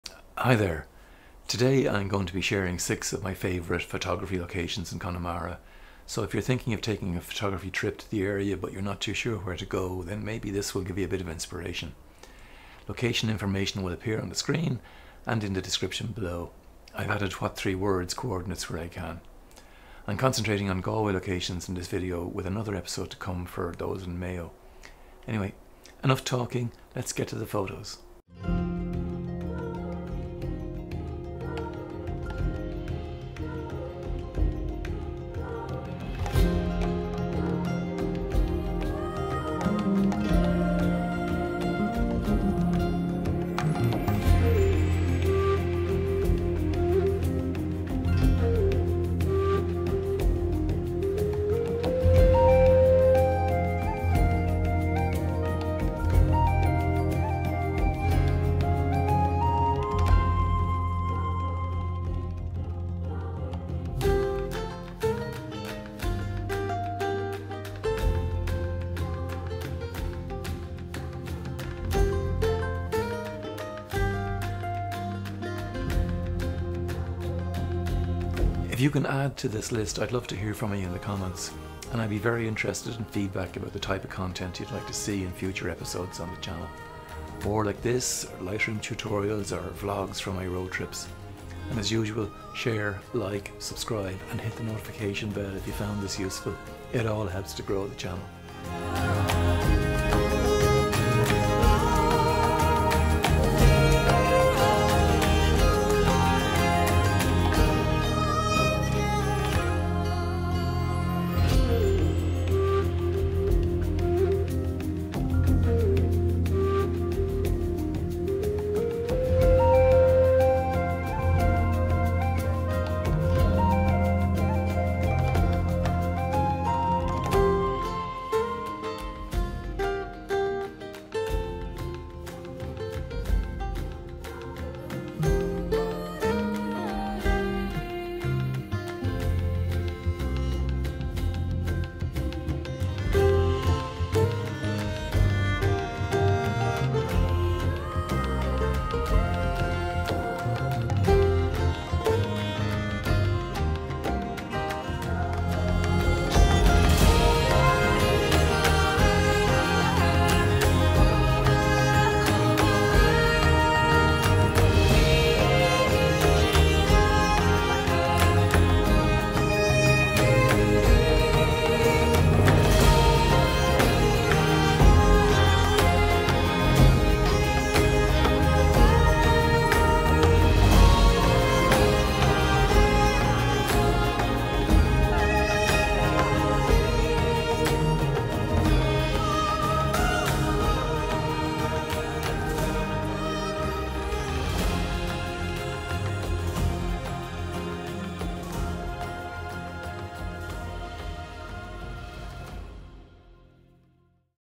Top 6 Connemara Photo Locations (Part 1 - Galway)
Part One of a two part piece about the top photo locations in Connemara. This episode focuses on the Galway side - Coming up next I'll be looking at locations in Mayo.

Locations using What 3 Words (What3words is a proprietary geocode system designed to identify any location on the surface of Earth with a resolution of about 3 metres)

Derryclare Lake / Pine Island ///mortgage.atoms.melons
Lough Inagh ///camera.petite.regionally
Near Lough Inagh Lodge ///aghast.shelves.varies
Roundstone Bog. //regulates.documenting.absurd
Screebe Waterfall and Gillie’s Hut ///scrabbling.tans.temperament
Screebe Fishermans Hut ///readers.maintenance.founders
Máméan ///toothpicks.quarrel.falling
R344 ///judge.brushed.speeders
Kylemore Abbey ///pubs.auction.aquariums
Kylemore Lake ///former.ignorance.admission
Killary Fjord ///patches.sipped.underworld